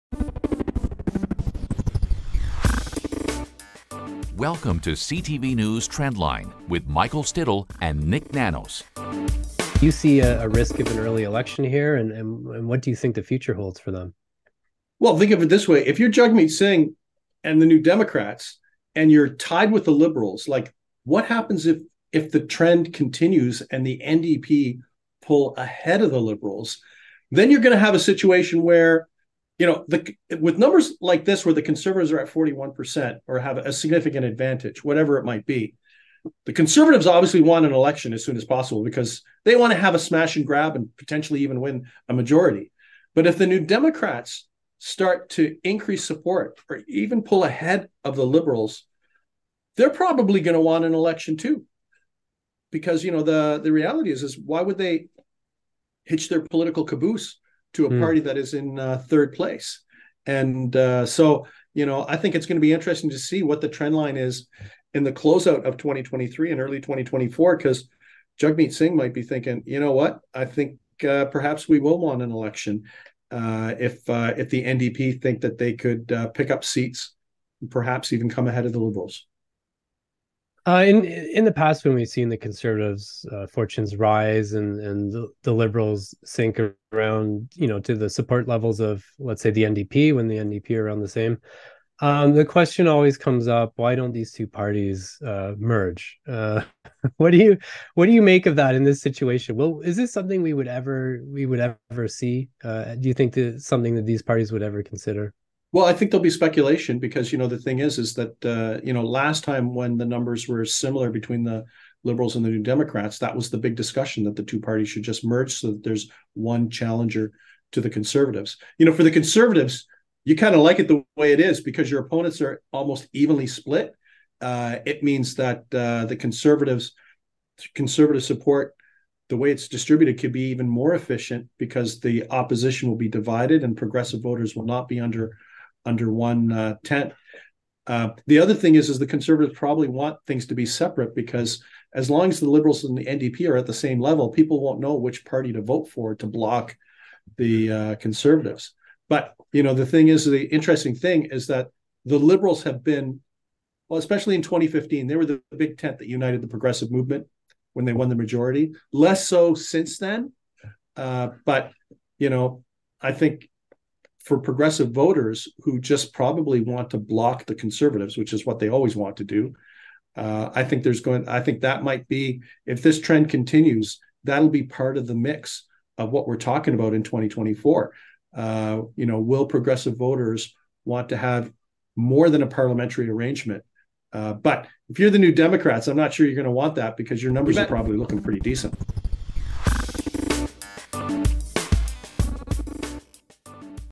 As Trudeau's polling drops, could Singh end Liberal-NDP deal? | TREND LINE
Trend Line's Michael Stittle and Nik Nanos break down new data on the Liberals and NDP. Should we expect an early election?